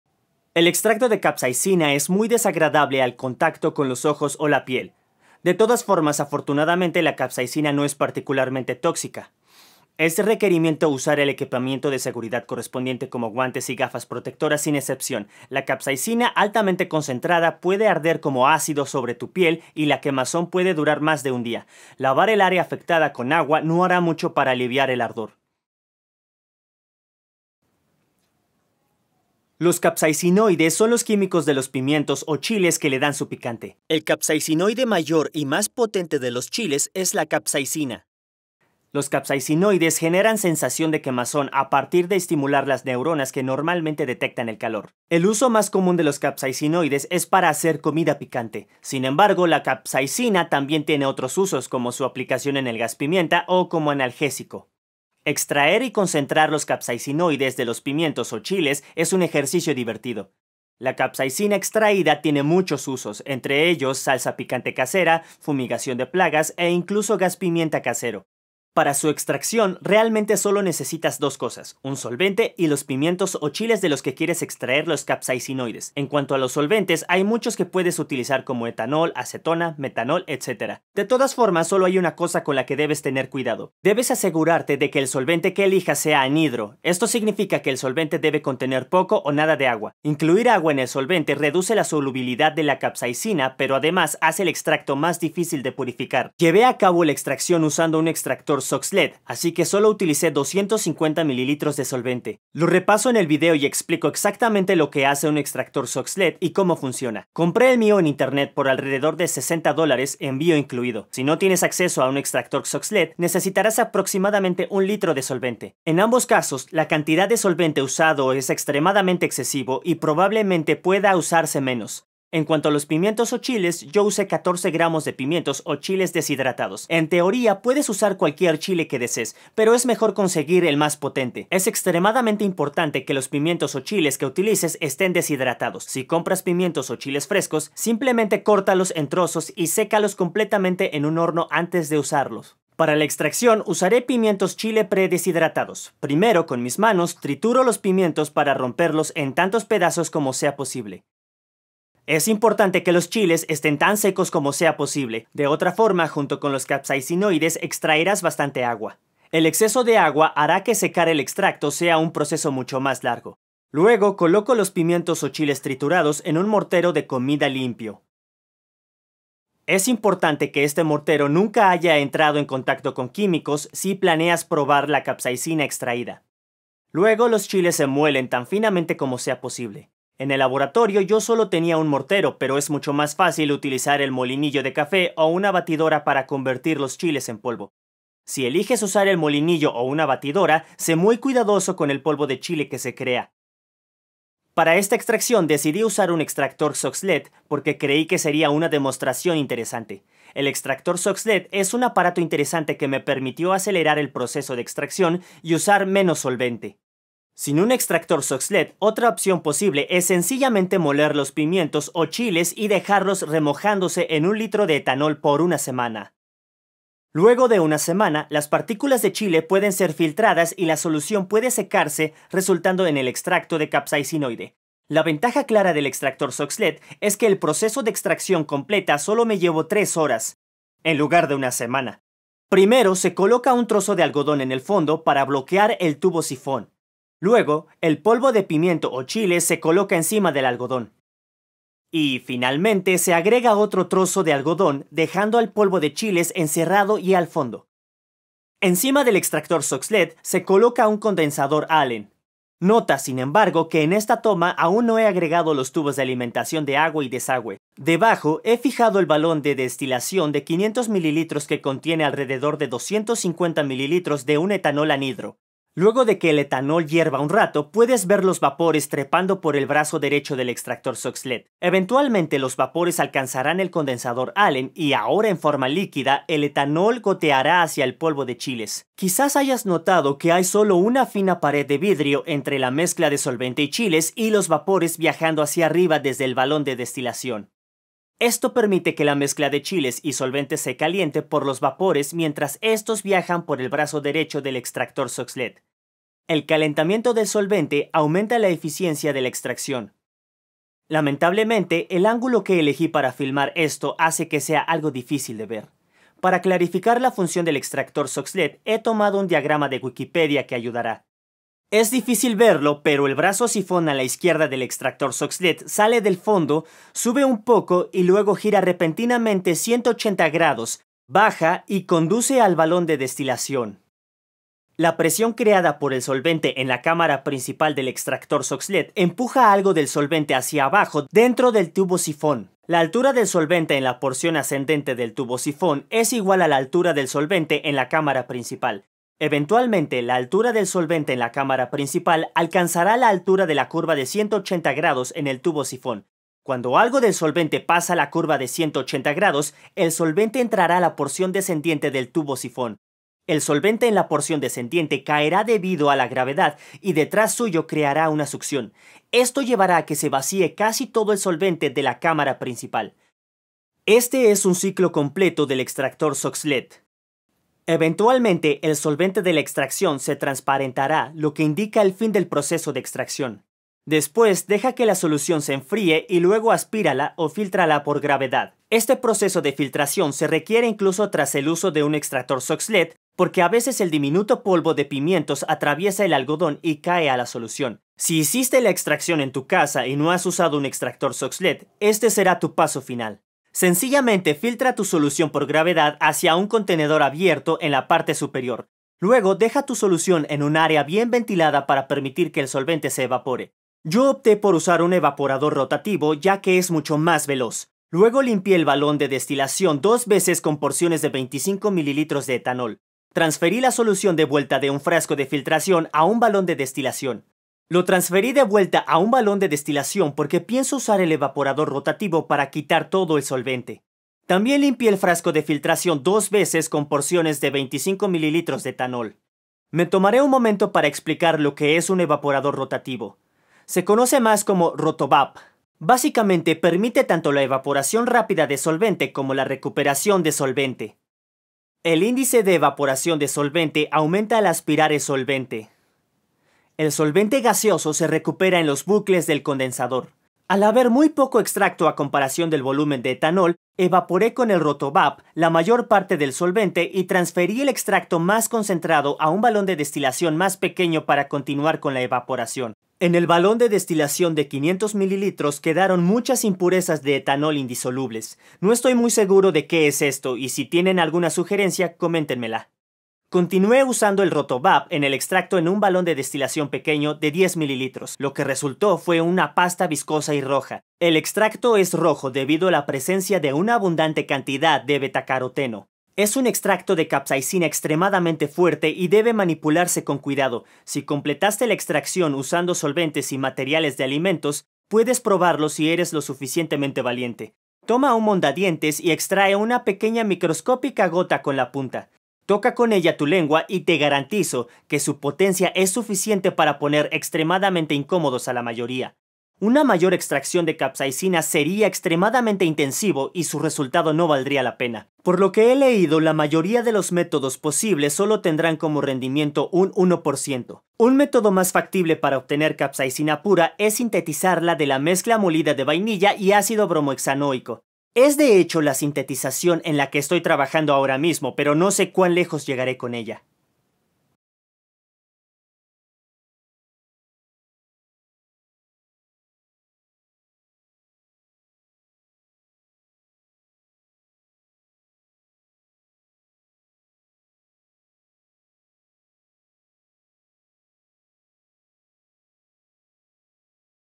Cómo extraer capsaicinoides de los chiles
Los capsaicinoides son compuestos interesantes con múltiples y muy diferentes aplicaciones.

En este video utilizo un extractor Soxhlet para extraer la capsaicina de chiles fantasma.